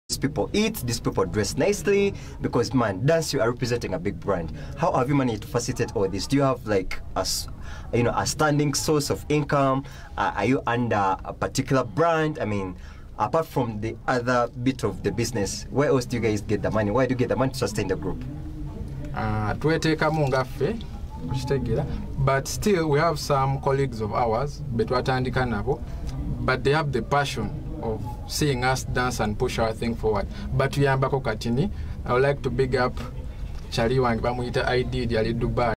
Interview with the C.O.B dancers
An exclusive interview of the journey of the C.O.B dancers live at COINCAST TV